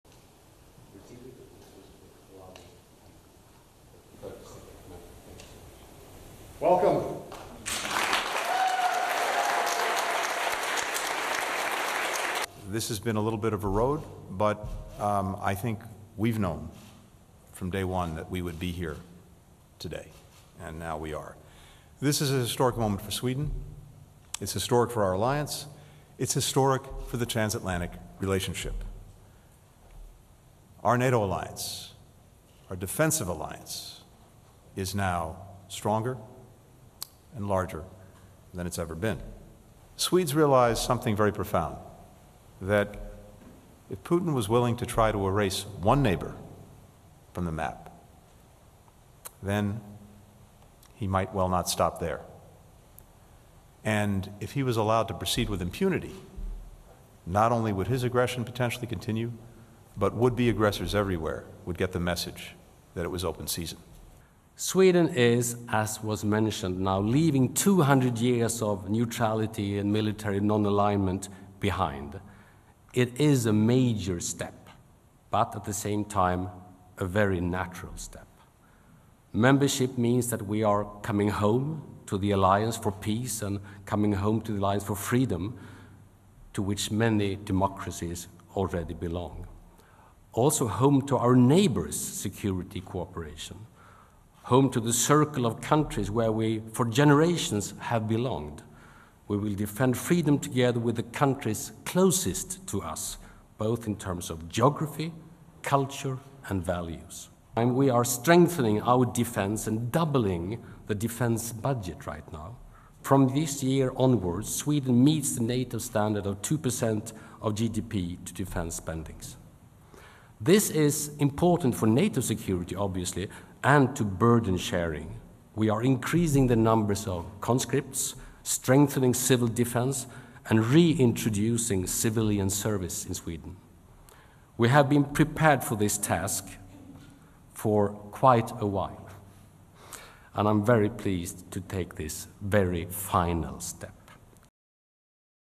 Sweden officially joins NATO, ending decades of post-World War II neutrality
Sweden has formally joined NATO as the 32nd member of the transatlantic military alliance, ending decades of post-World War II neutrality.